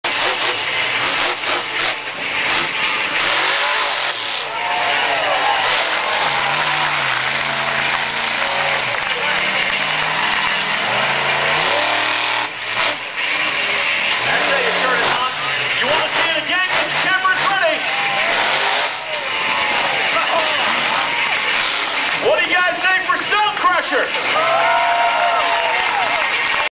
Stonecrusher Wheelie contest at Stafford springs CT
This is Steve Sims driving Stonecrusher at Stafford Springs CT